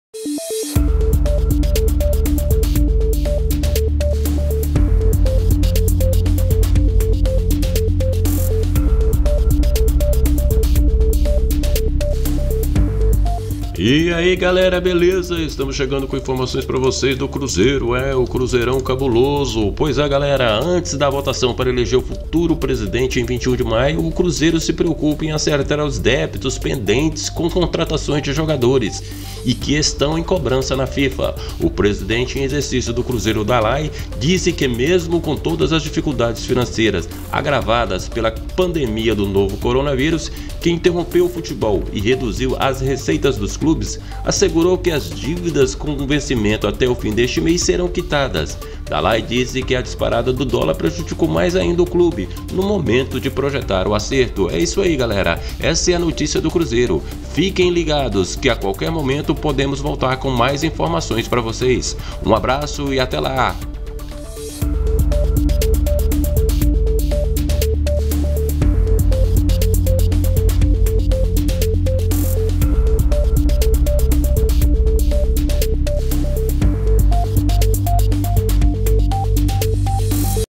√ Cruzeiro I Dalai garante pagamento de débitos na Fifa I 14.05.2020
√CRUZEIRO

Presidente interino do Cruzeiro garante pagamento de débitos na Fifa
Dalai lamenta valorização do dólar, mas assegura quitação.

Antes da votação para eleger o futuro presidente, em 21 de maio, o Cruzeiro se preocupa em acertar os débitos pendentes com contratações de jogadores e que estão em cobrança na Fifa. O presidente interino do clube, José Dalai Rocha, participou de uma live na internet e garantiu que os valores com vencimento até o fim deste mês serão quitados.

O presidente em exercício do Cruzeiro, mesmo com todas as dificuldades financeiras, agravadas pela pandemia do novo coronavírus, que interrompeu o futebol e reduziu as receitas dos clubes, assegurou que as dívidas com vencimento até o fim deste mês serão quitadas.

“Temos caminhos a serem testados e é uma coisa dificílima, pois envolve recursos e o país está parado, as empresas estão paradas. Estamos sofrendo tudo isso, mas vamos conseguir resolver esses graves assuntos”, acrescentou o mandatário, garantindo que o Cruzeiro quitará ‘As (dívidas) que vencerem em maio, seguramente, com todo sacrifício do conselho gestor.'

Dalai concedeu entrevista ao filho, o jornalista Fernando Rocha, por meio de live no Instagram. Entre outros assuntos, o presidente interino do Cruzeiro foi perguntado sobre o valor da dívida cobrada na Fifa e o mandatário disse que, além da quantidade de débitos pendentes, a dificuldade é maior por causa da valorização do dólar sobre o Real. A moeda norte-americana fechou esta quarta-feira cotada a R$ 5,89, o que aumenta o total a ser quitado em contratações anteriores.

“Tive a curiosidade de anotar e ver a dívida da FIFA. O Denílson, volante comprado (na verdade foi empréstimo) aos Emirados Árabes (Al Whada) por R$ 5 milhões e R$ 6 milhões, o Willian Bigode, comprado ao Zorya, da Ucrânia, por R$ 11 milhões. E o Caicedo foi comprado ao Del Valle, aquele cara que errava quase todas as bolas, devemos quase R$ 4 milhões. Hoje, o total é de aproximadamente R$ 20 milhões, R4 21 milhões. Há pouco tempo, eram R$ 10 milhões”, ressaltou o dirigente.

Dalai disse que a disparada do dólar prejudicou mais ainda o clube no momento de projetar o acerto. “O Cruzeiro teve a surpresa com o valor excessivo do dólar, que beira os R$ 6. A dívida, então, subiu demais. Estamos administrando essas coisas de cinco, seis anos atrás, que caiu no nosso colo. Assumimos a solução disso”, declarou.

Fifa,
Dividas
Quitação.
Contratações de Jogadores,